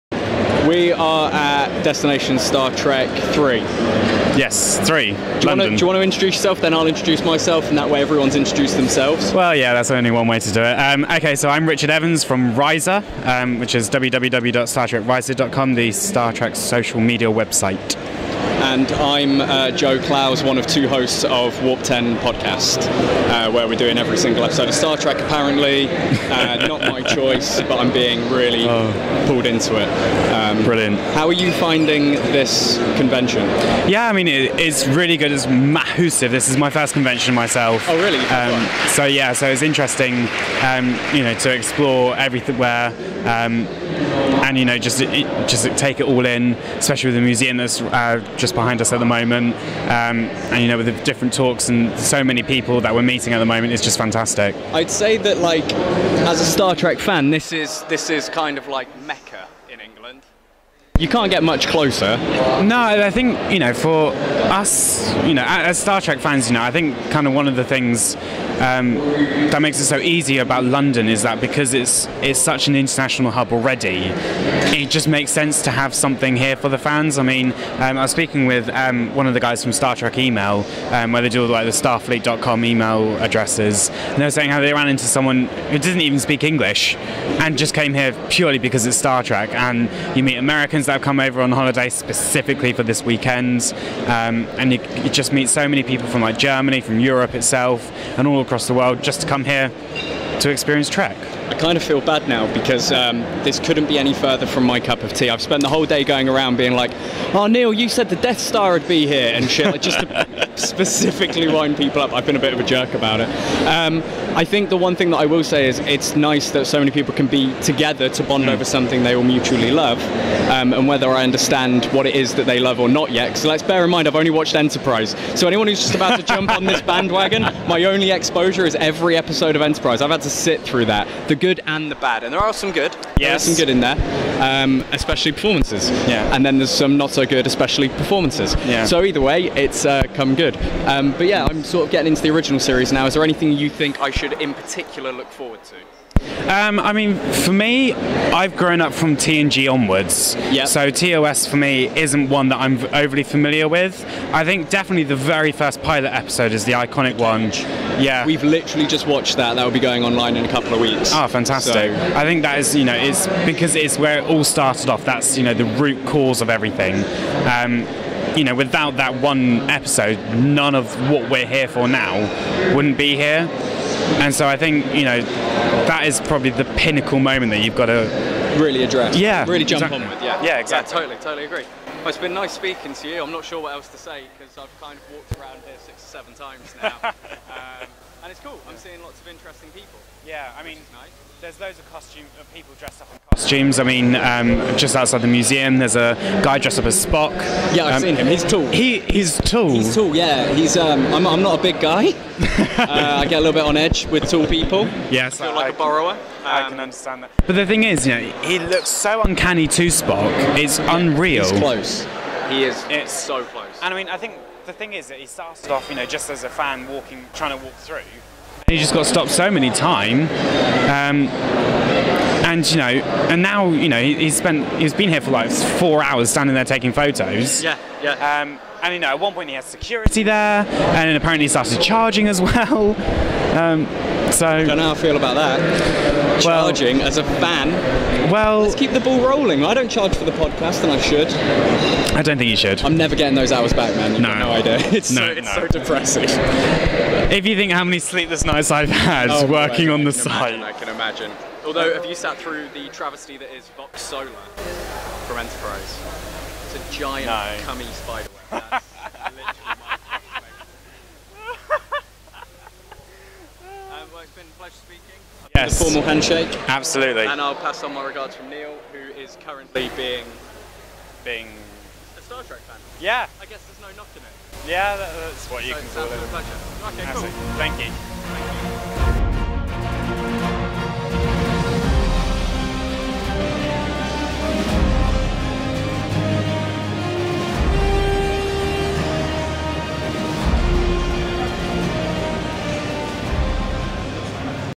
Risa & Warp 10 Podcast
Richard from Star Trek Rise and Joe from Warp 10 Podcast got together for a chat at Destination Star Trek 3

Credits:
Camera: John Fisher
Additional Music: I am not left handed
jfmediastudios.co.uk